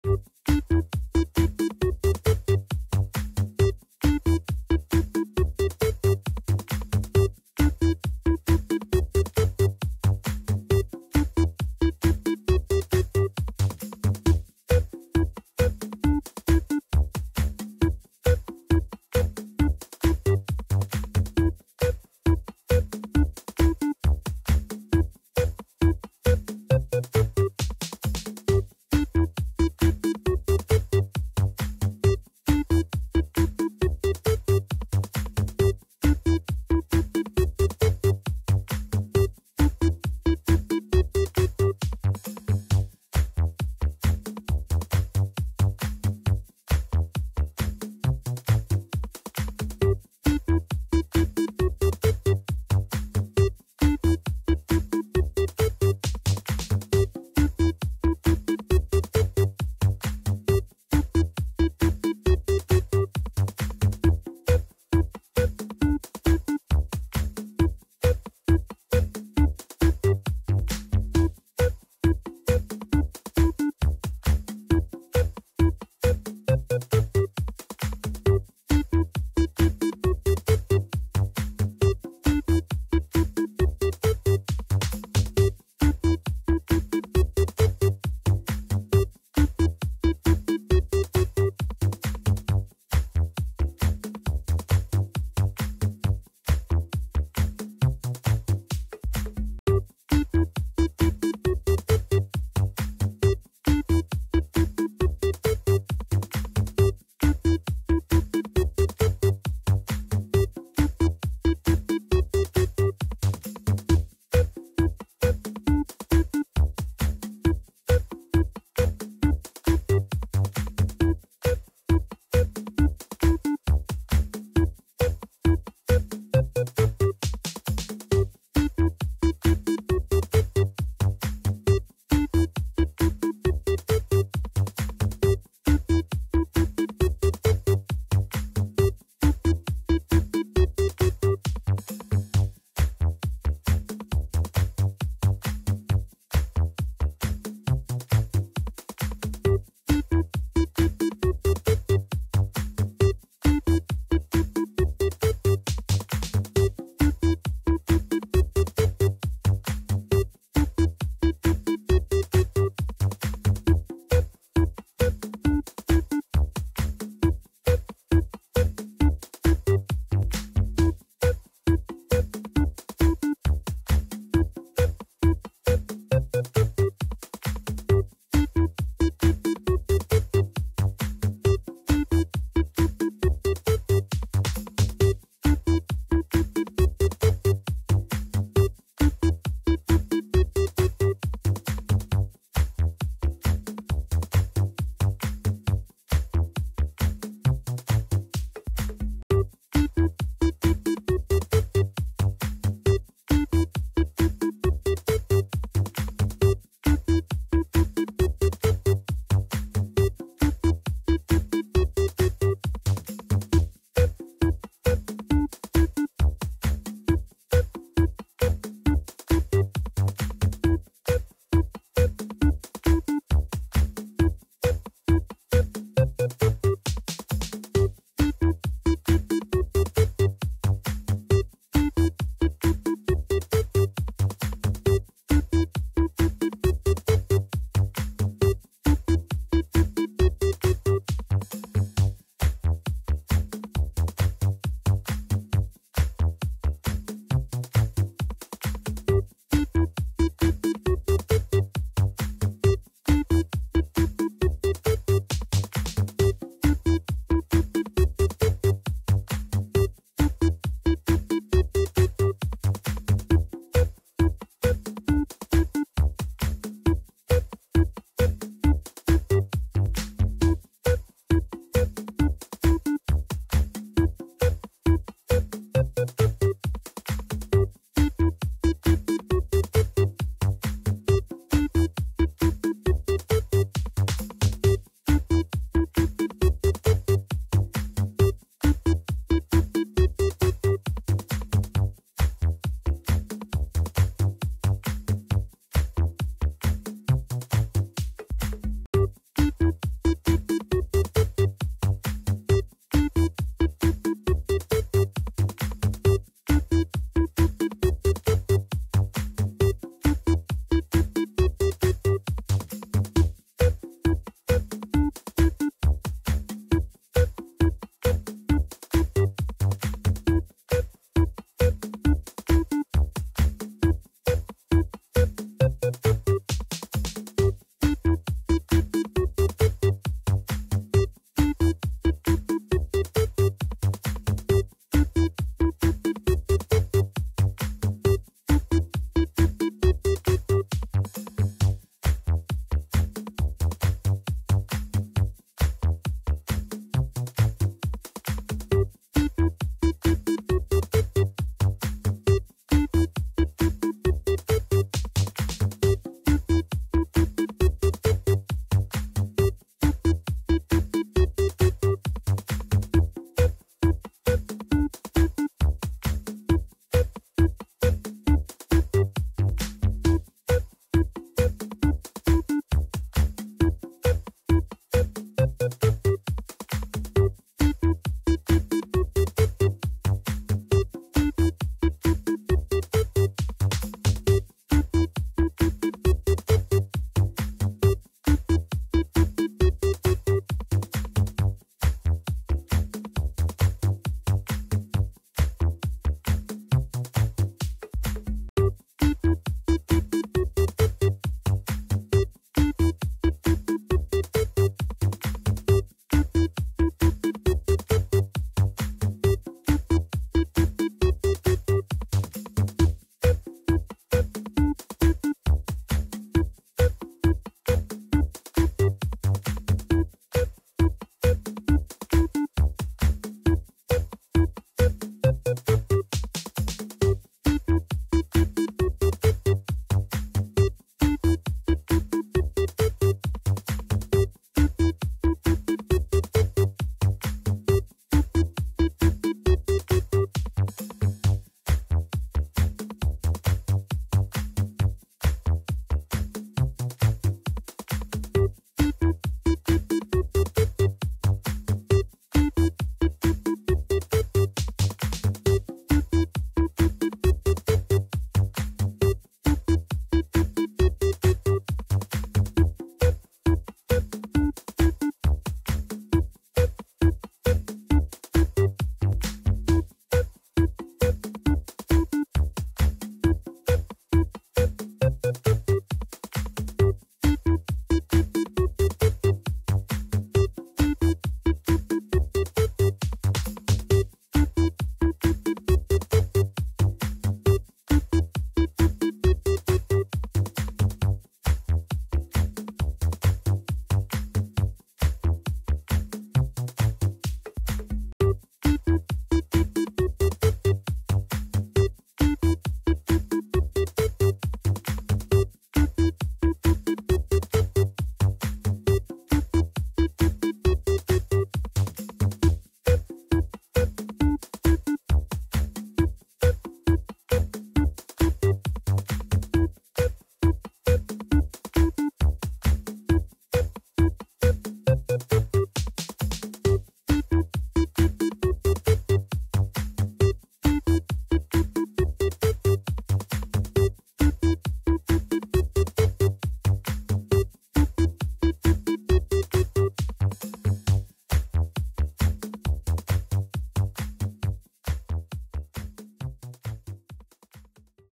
【折り紙】まめっち（たまごっち）の作り方
折り紙でたまごっちの「まめっち」をつくります。